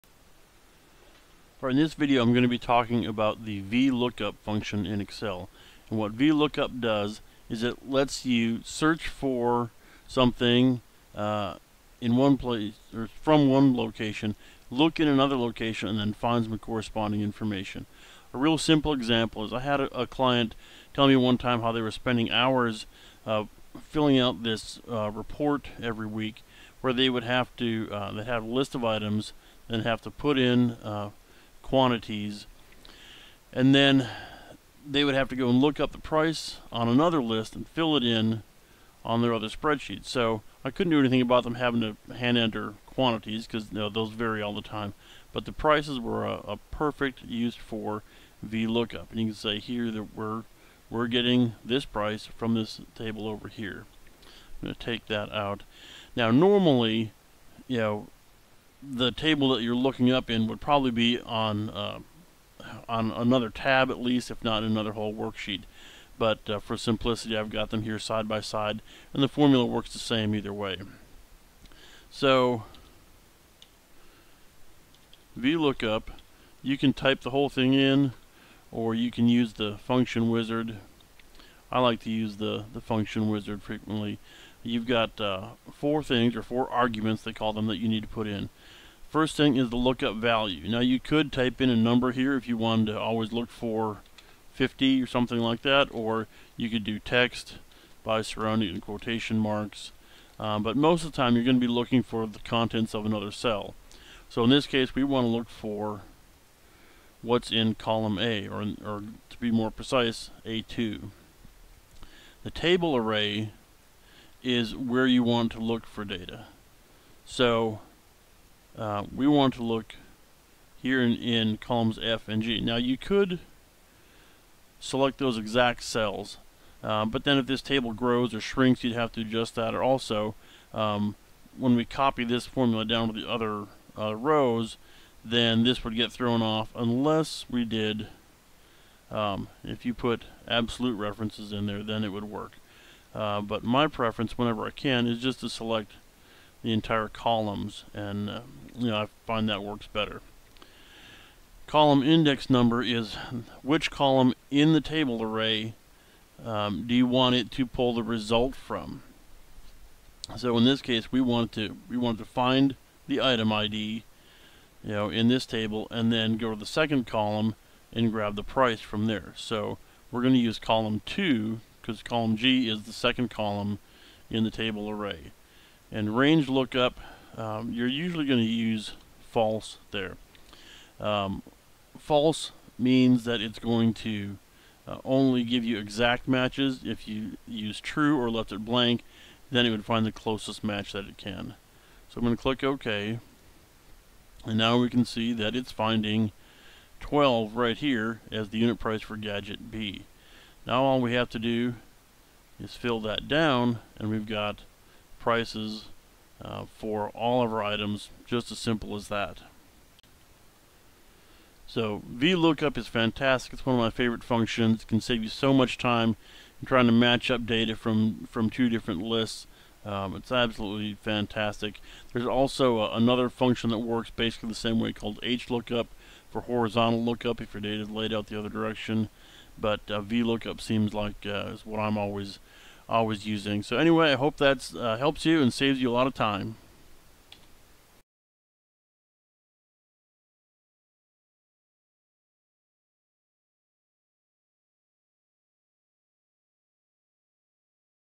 Using VLOOKUP in Excel to Find Data In a List
Excel's VLOOKUP function makes it easy to combine data from to locations. You can search for a match in one column of a table and return a result from the same line of another column.